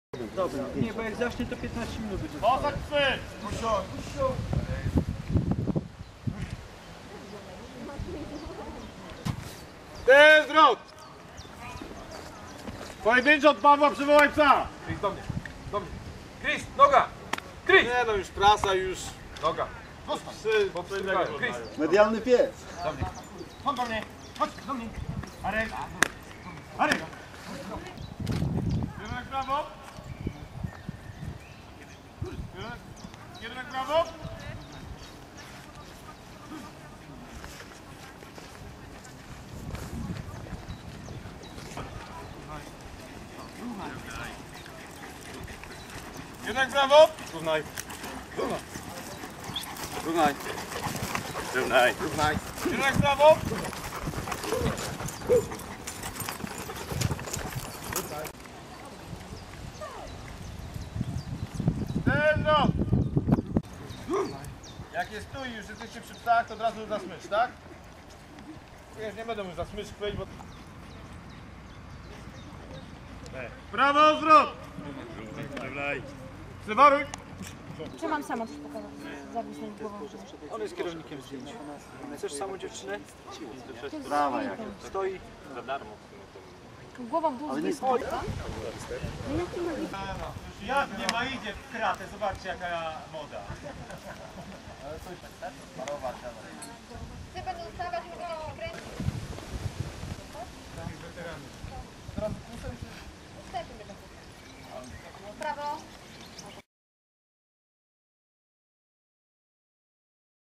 Policyjne psy i konie
Policjanci oswajają psy i konie, żeby mogły razem pracować przy zabezpieczaniu imprez masowych.